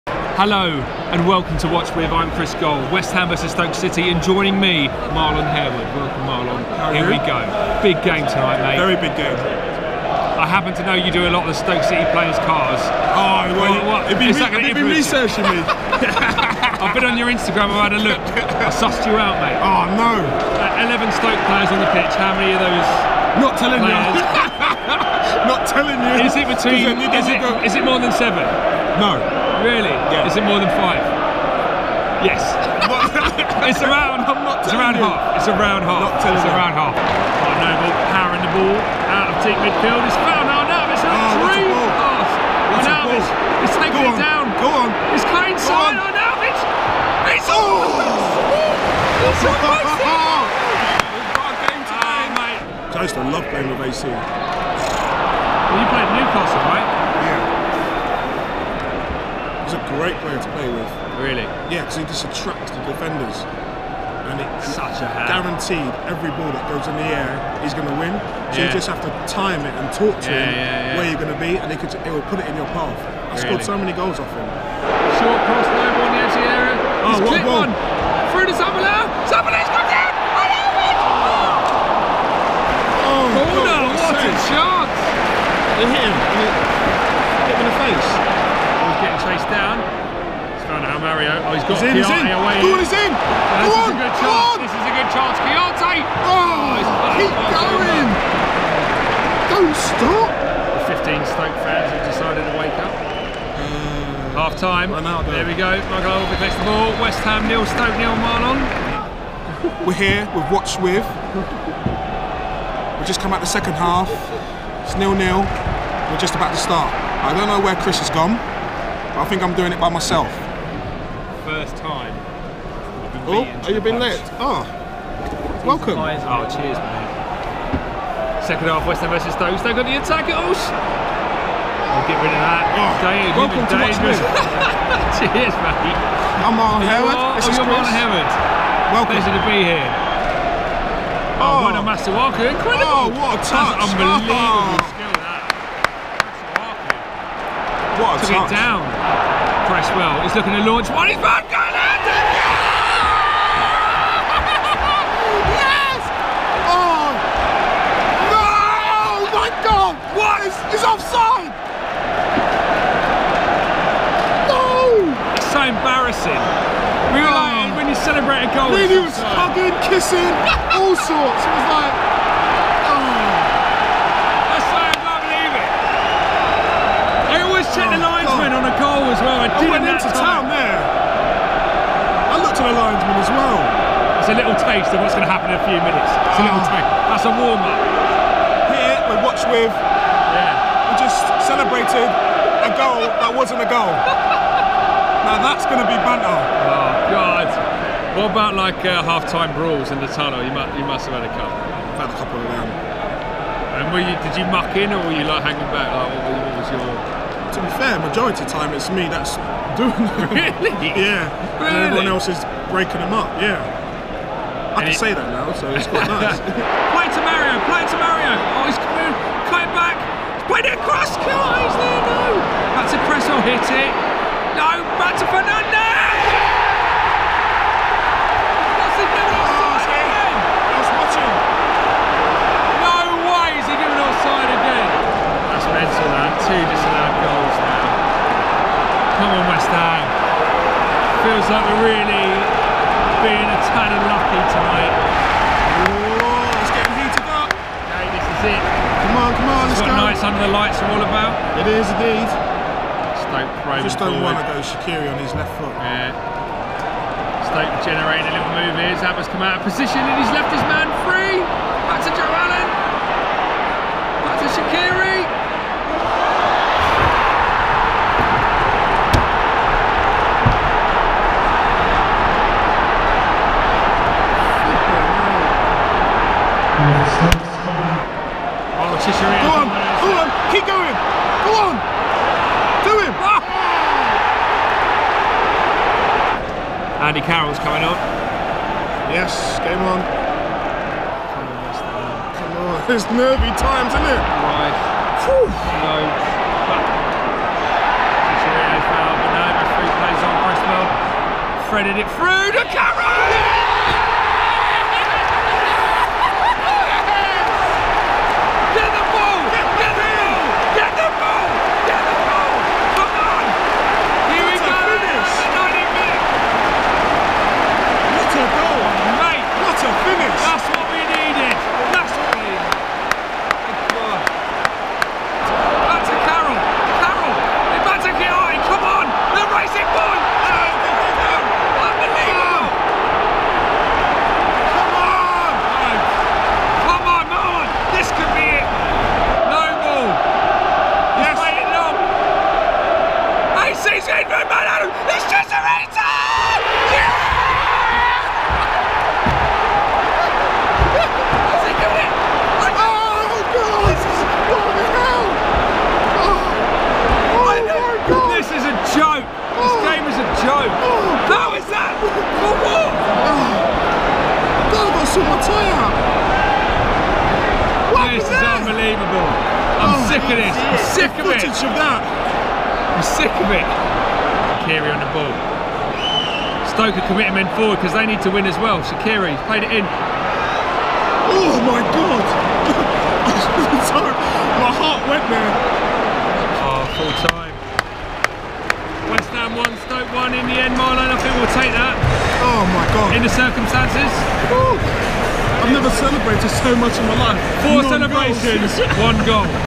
WATCH WITH | 4 CELEBRATIONS, 1 GOAL!? HAREWOOD CAN'T BELIEVE IT...
Monday's draw with Stoke was certainly not lacking in drama! Harewood joins Chris for an emotional rollercoaster of a game!